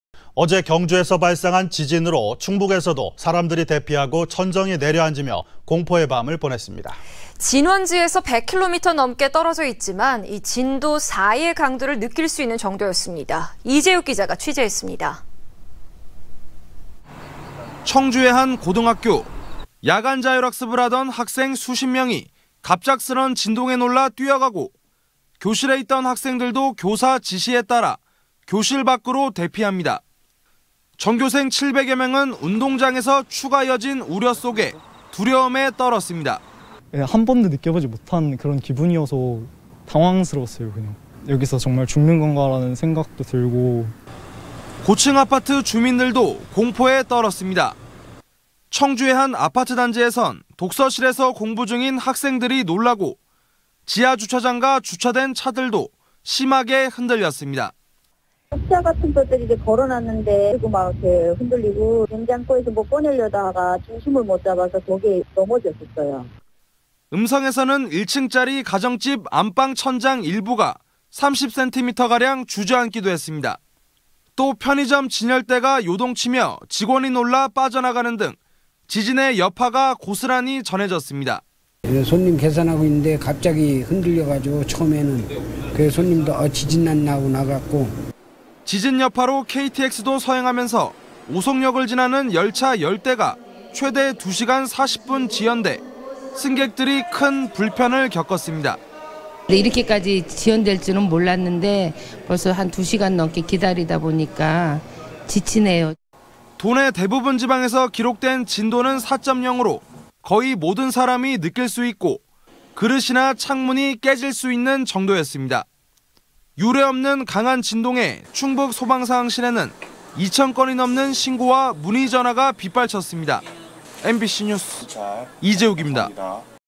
[청주MBC뉴스] '역대 최강 지진' 충북도 공포의 밤
◀앵커▶
 어제(12) 경주 지진의 여파로 충북에서도
사람들이 대피하고 천정이 내려앉는 등
공포의 밤을 보냈습니다.
 진원지에서 100km 넘게 떨어져 있지만
진도 4의 강도를 느낄 수 있는 정도였습니다.
           ◀ＥＮＤ▶

           ◀ＶＣＲ▶
 청주의 한 고등학교.

 야간자율학습을 하던 학생 수십 명이
갑작스런 진동에 놀라 뛰어가고
교실에 있던 학생들도 교사 지시에 따라
교실 밖으로 대피합니다.

 전교생 7백여 명은 운동장에서
추가 여진 우려 속에 두려움에 떨었습니다.

     ◀ＩＮＴ▶ 이시연/고등학생
  "당황스러웠다. 죽는 거 아니냐는 생각도"

 고층 아파트 주민들도 공포에 떨었습니다.

 청주의 한 아파트단지에선
독서실에서 공부중인 학생들이 놀라고
지하주차장과 주차된 차들도
심하게 흔들렸습니다.

       ◀ＳＹＮ▶ 류영실/가경동
     "국자 흔들, 냉장고 닦다 넘어져"

 음성에서는 1층짜리 가정집 안방
천장 일부가 30cm 가량 주저앉기도 했습니다.

 또 편의점 진열대가 요동치며
직원이 놀라 빠져나가는 등
지진의 여파가 고스란히 전해졌습니다.

   ◀ＩＮＴ▶ 황태연/편의점 주인
    "문도 흔들리고 손님도 놀라고"

 지진 여파로 KTX도 서행하면서 오송역을
지나는 열차 10대가 최대 2시간 40분 지연돼
승객들이 큰 불편을 겪었습니다.

   ◀ＩＮＴ▶ 정임순 송라희/문의면
    "2시간 동안 기다리다 완전 지쳤다"

 도내 대부분 지방에서 기록된 진도는
4.0으로 거의 모든 사람이 느낄 수 있고
그릇이나 창문이 깨질 수 있는 정도였습니다.

 유래없는 강한 진동에 충북소방상황실에는
2천 건이 넘는 신고와 문의전화가
빗발쳤습니다. MBC뉴스 이재욱입니다.
           ◀ＥＮＤ▶